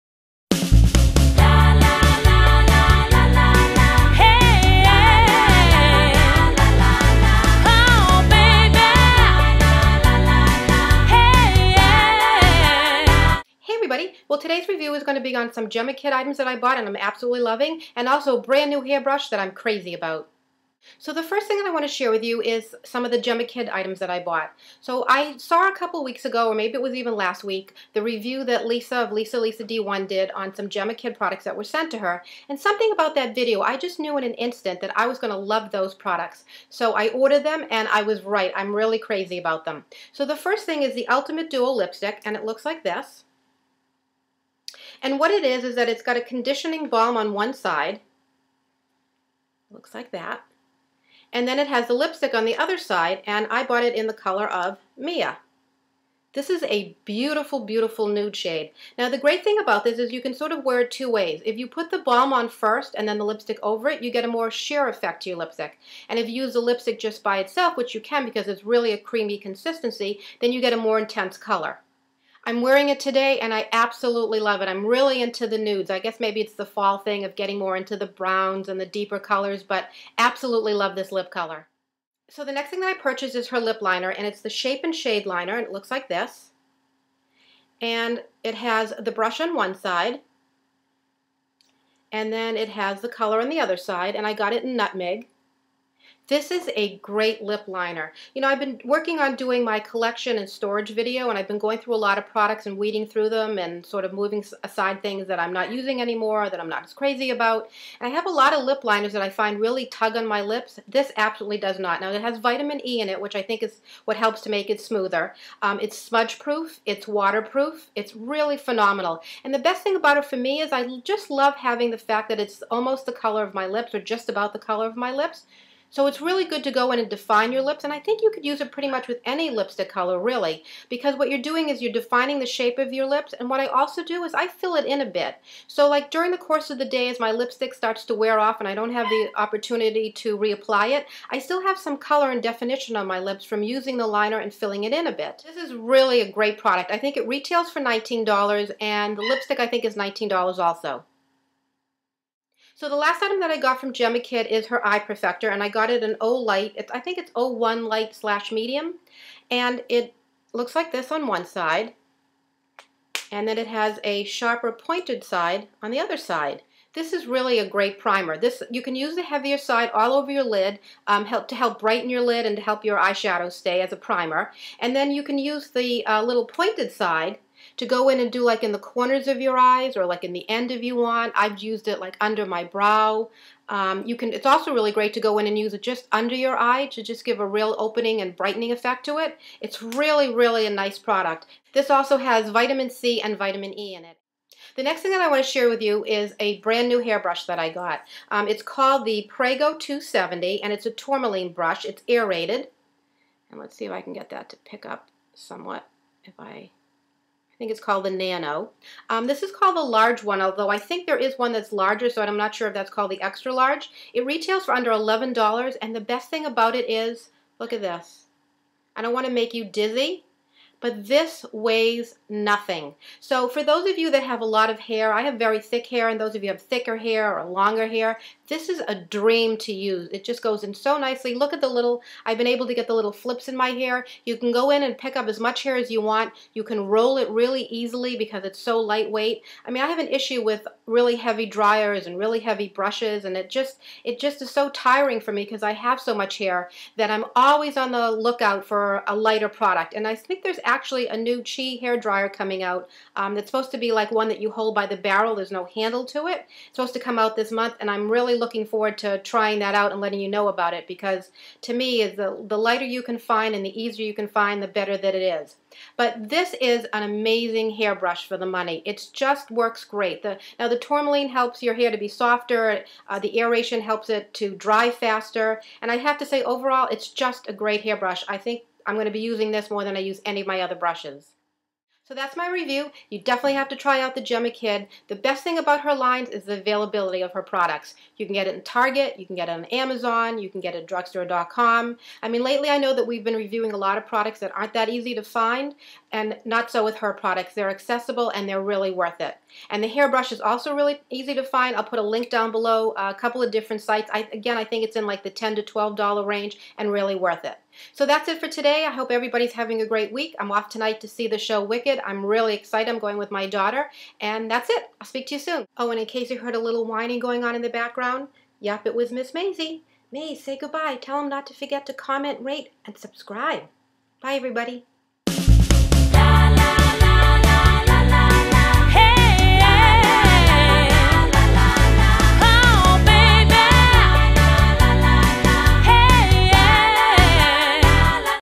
Jemma Kid & New Hair Brush
Jemma Kidd & A Great New Hair Brush

Jemma Kidd Pro I-Perfector 01
Jemma Kidd Ultimate Duo Lipstick in Mia
Jemma Kidd Shape & Shade Liner/Lip Fill in Nutmeg